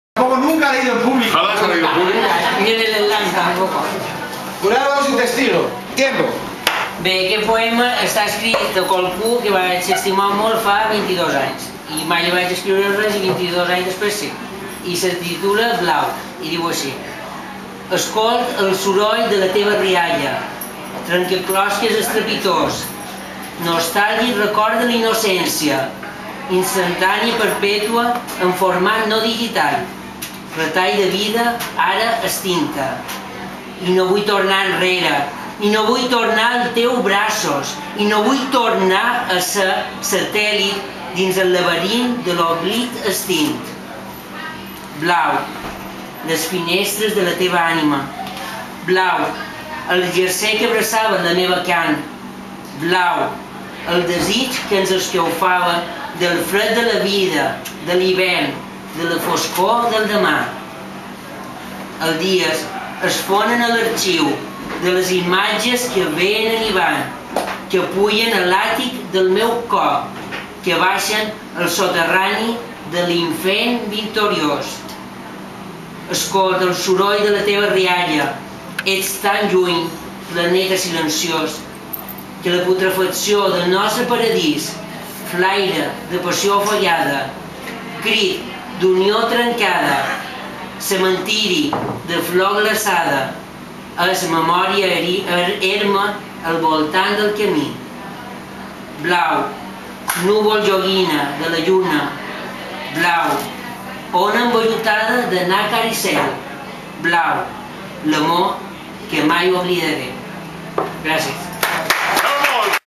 Jaume Gaviño lee el primer poema en el slam poético de l' Antiquari
Jaume Gaviño lee el primer poema en el slam poético de l' Antiquari, 15 de septiembre de 2010.